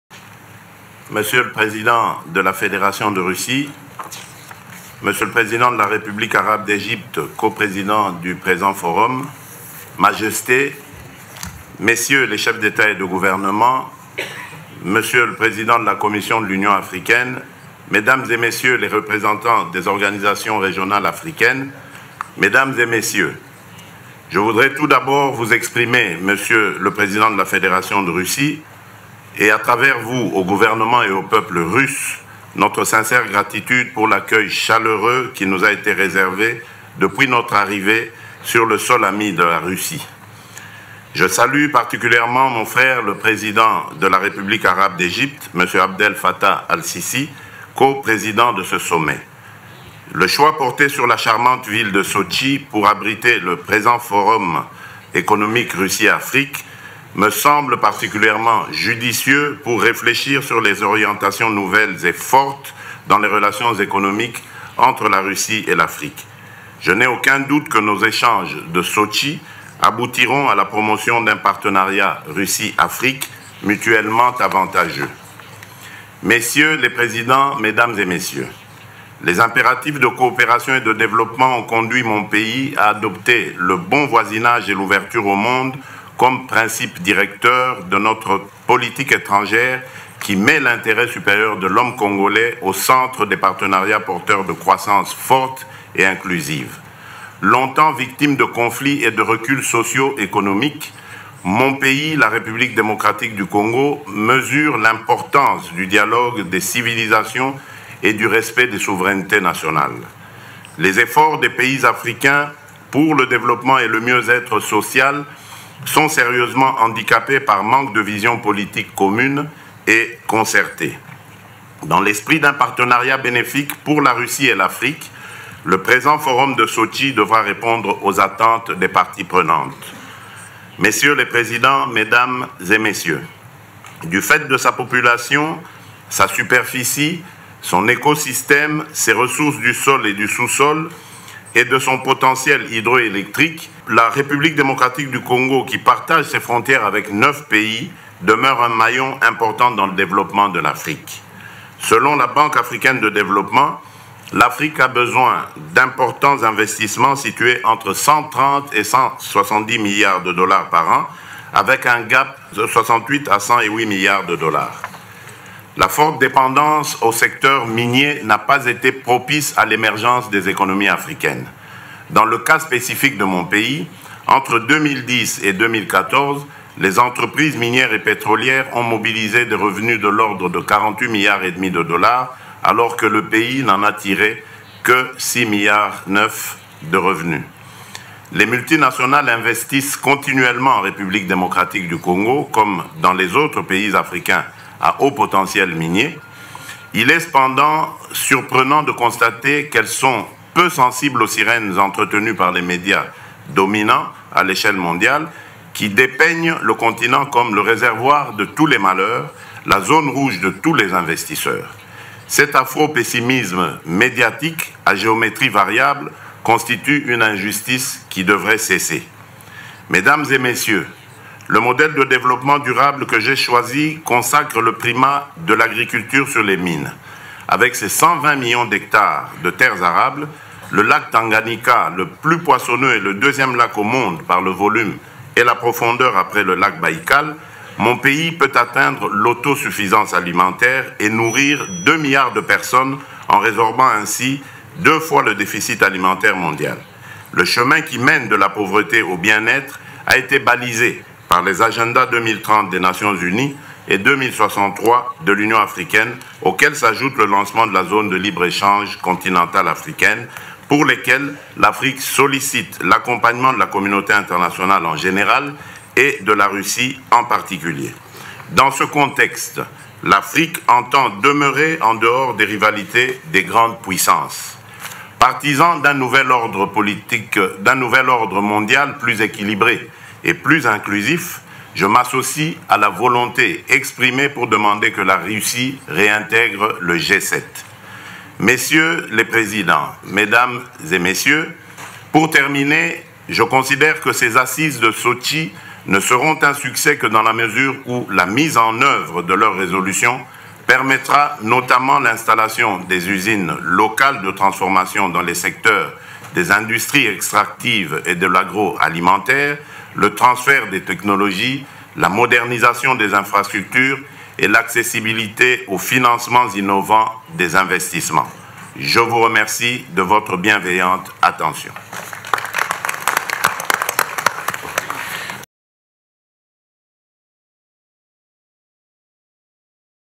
Premier Forum Russie Afrique
24 octobre 2019
Adresse du Président Félix Tshisekedi faite lors de la seconde journée du sommet Russie-Afrique
Partenariat Russie Afrique en marche